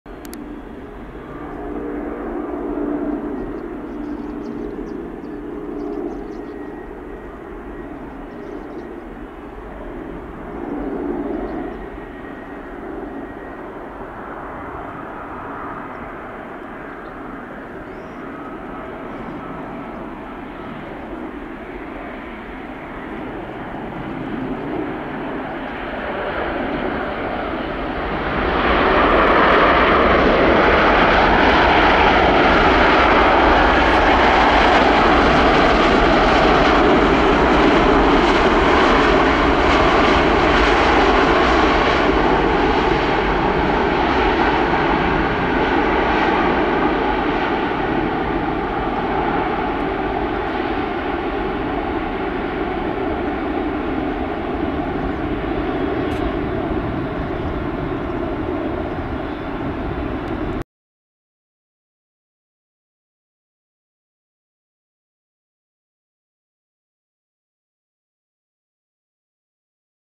Airbus A350-1041 lands at YVR
British Airways Airbus A350-1041 lands in Vancouver.